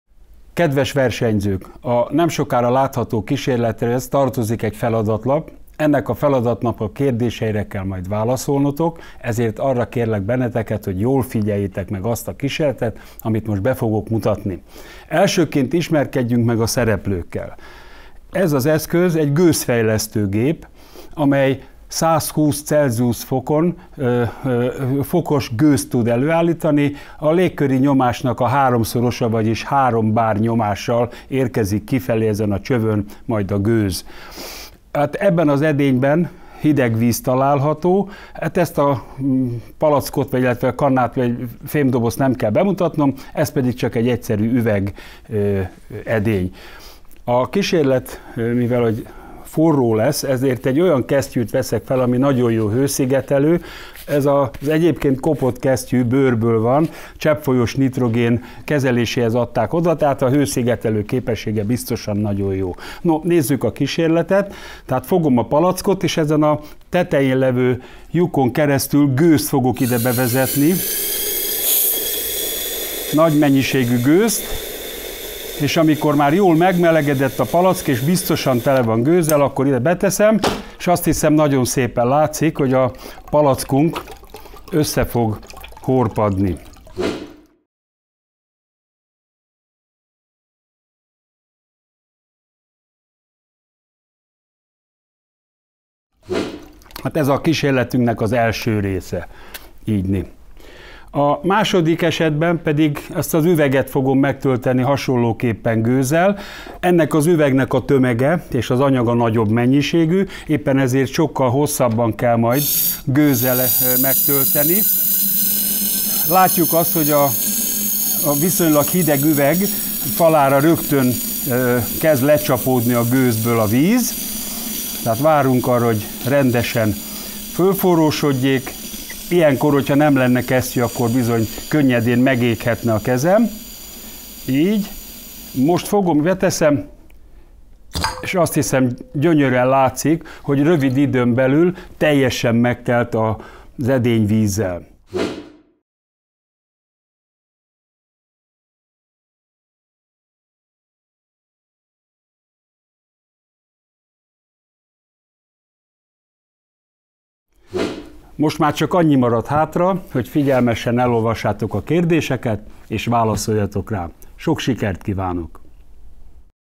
ÖVEGES JÓZSEF KÁRPÁT-MEDENCEI FIZIKAVERSENY kísérletelemző feladat 2021
A versenyt az Eötvös Loránd Fizikai Társulat Általános Iskolai Oktatási Szakcsoportja rendezi
A verseny honlapja: